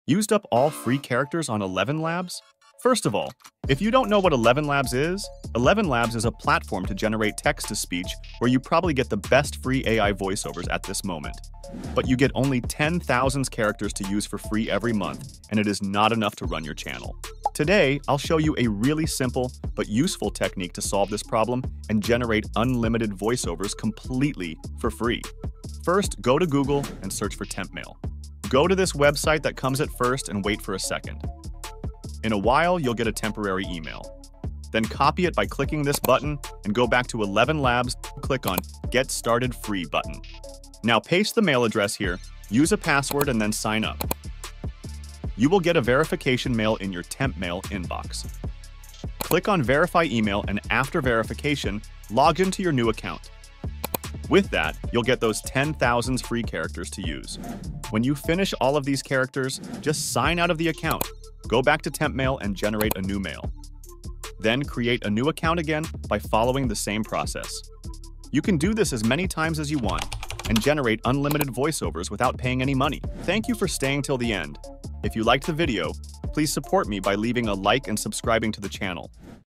How to use Elevenlabs for free forever (working 2025)
How to use Elevenlabs for free forever!

Unlock the power of Eleven Labs for free forever! Learn step-by-step how to access and use Eleven Labs AI voiceover generator without any cost. Follow this tutorial for unlimited, free usage and make the most of Eleven Labs' incredible features. Say goodbye to trial limitations and embrace the full potential of Eleven Labs! 🚀